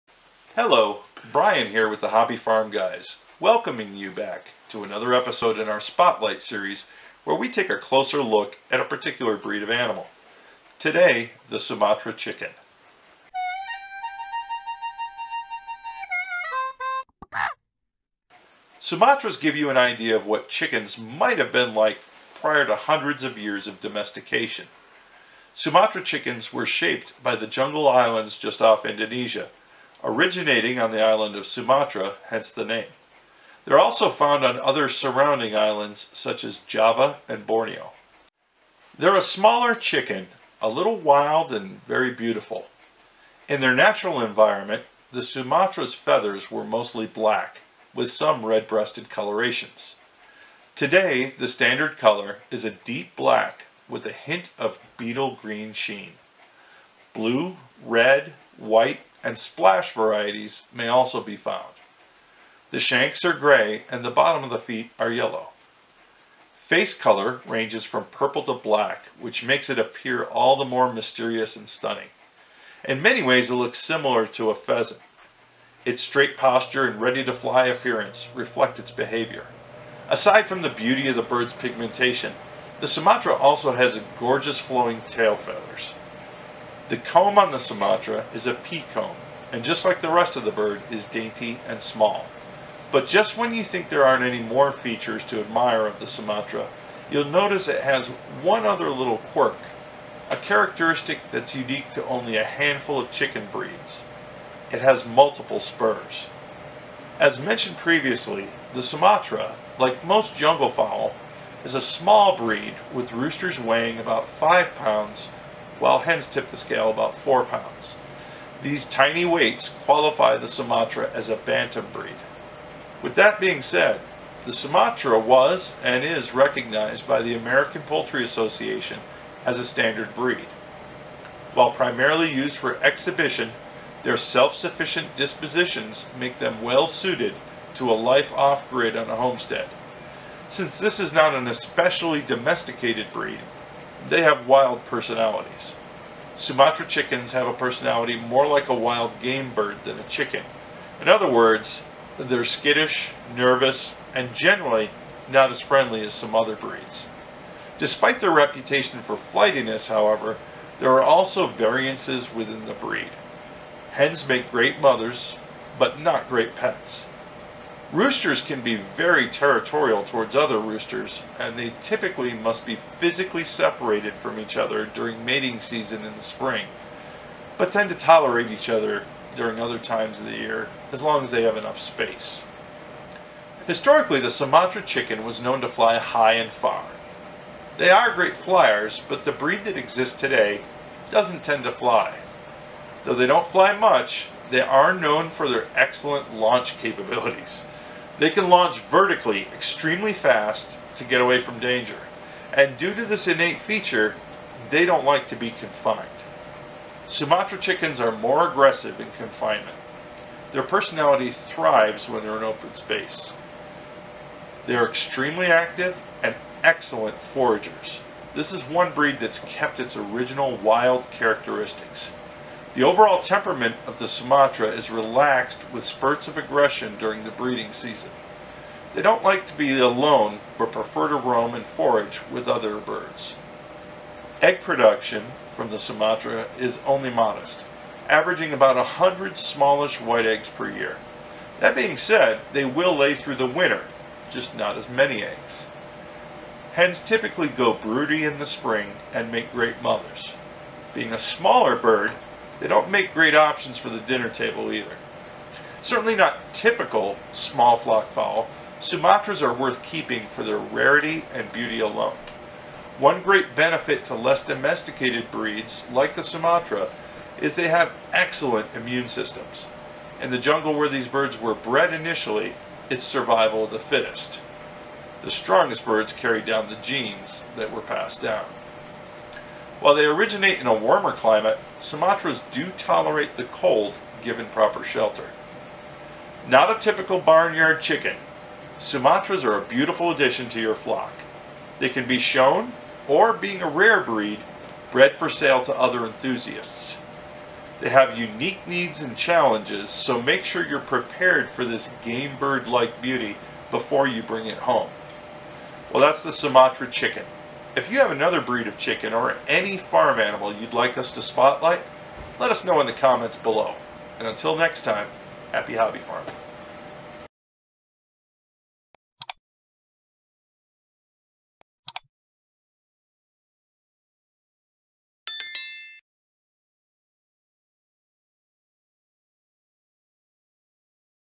HFG Farm Animal Spotlight: Sumatra Chickens
An unusual breed of chicken not often seen in backyard flocks. This semi-domestic breed is a real show off, and Brian gives you the details about this unique and stunning bird.